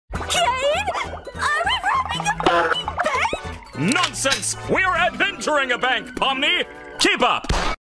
Caine and Pomni rob a bank‼️
Just a silly little shitpost I made 👁️👄👁️. . . Caine can be a bit crazy Lmao